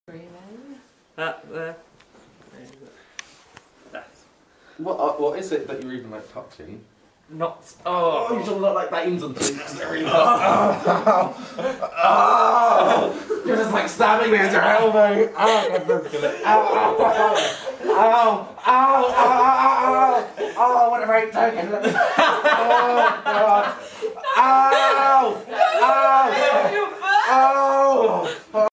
Joe's attempt to give Seb a massage
Seb screams like a mad little girl :)